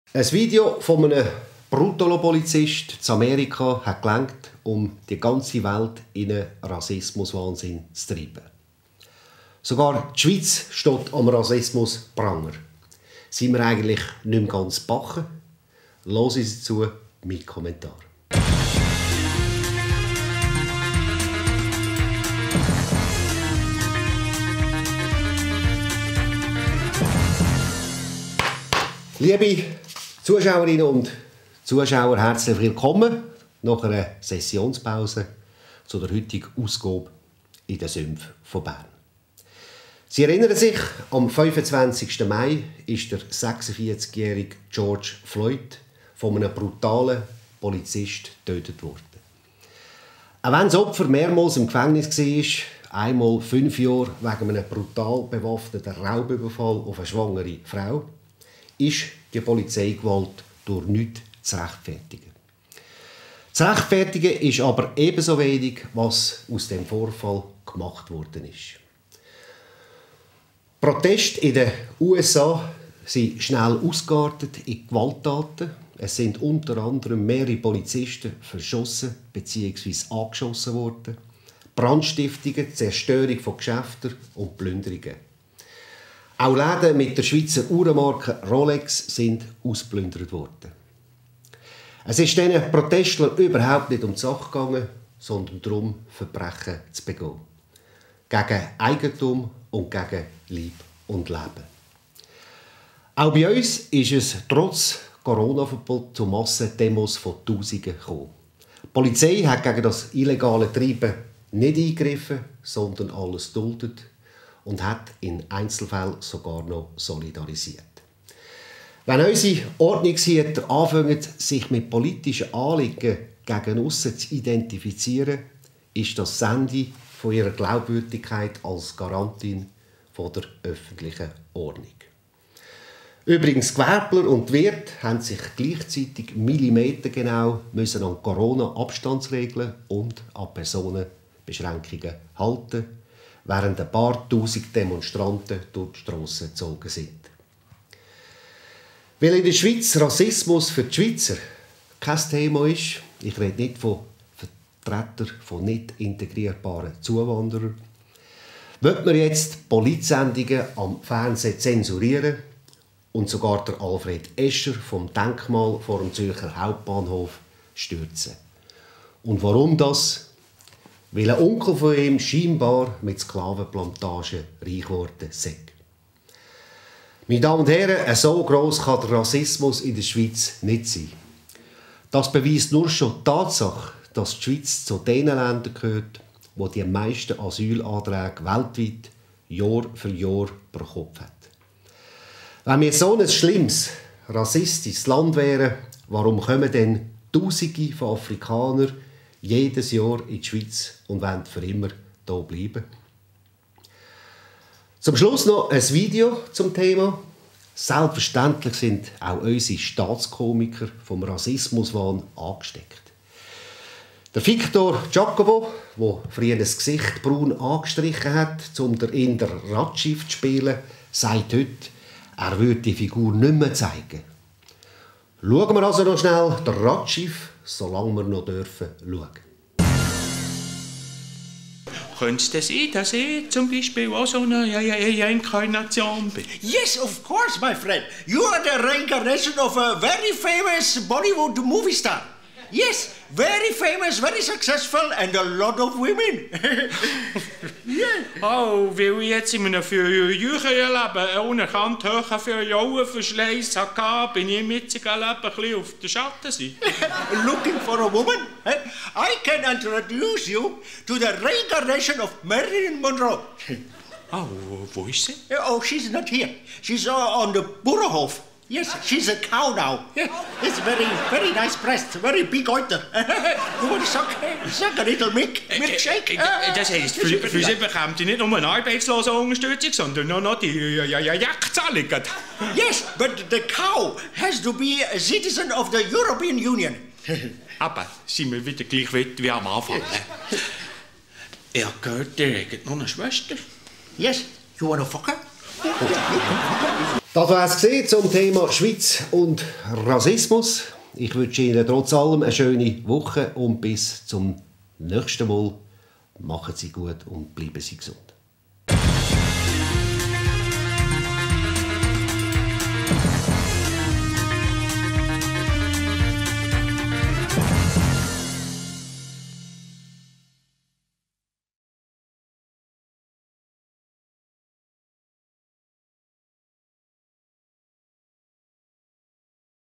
Rassismus-Hysterie
Ein Video von einem Brutalo-Polizist in Amerika hat gereicht, um die ganze Welt in einen Rassismus Wahnsinn zu treiben. Sogar die Schweiz steht am Rassismus-Pranger. Sind wir eigentlich nicht mehr ganz bei Sinnen? Teilen Sie mir Ihre Gedanken zu diesem Thema in der Kommentarspalte mit. Ich bin gespannt!

Abbonieren Sie meinen Podcast auf Spotify, Apple Podcasts und Google Podcasts!